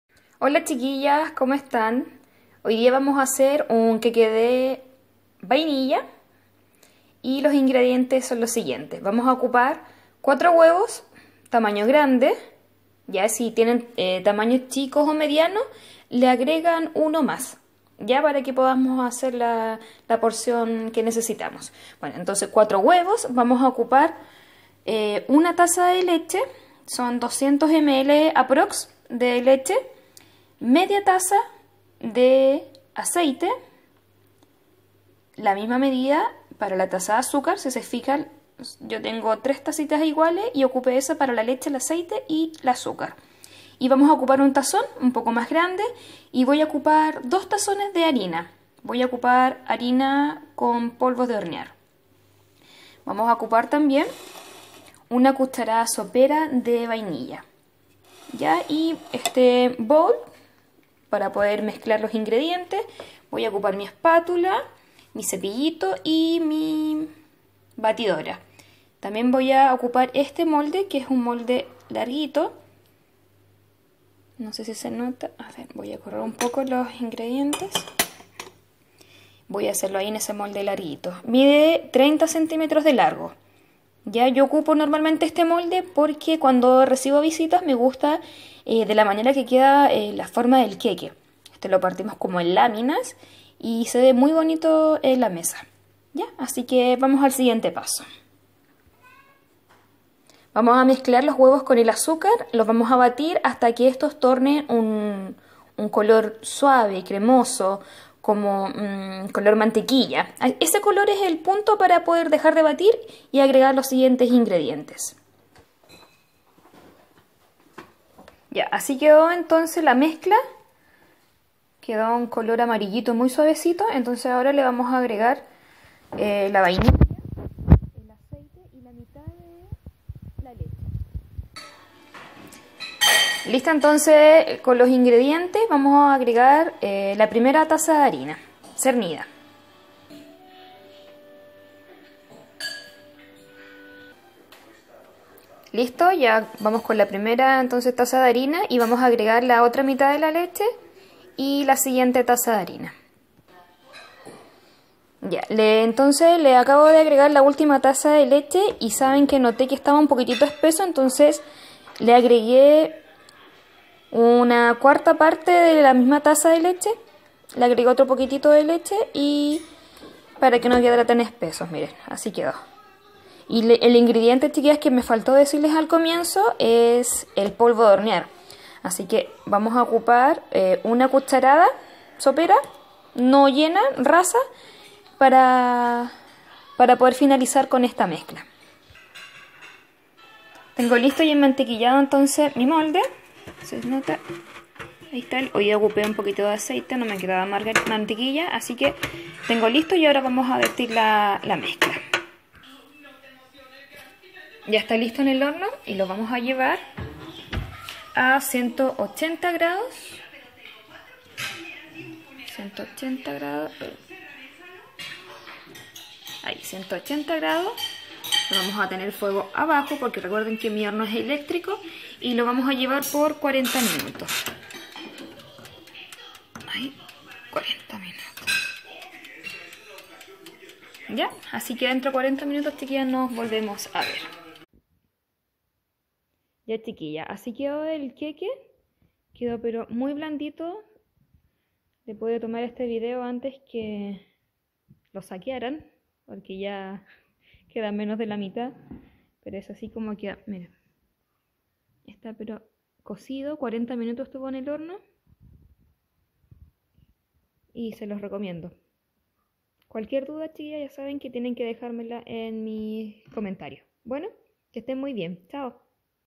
Queque de Vainilla
ingredientes:
2 tazones de harina c/p
1taza de azúcar
1taza de leche
1/2 taza de azúcar
4huevos
1cucharada de polvo de hornear
1cucharada de vainilla

cocción :
40minutos al horno
180 grados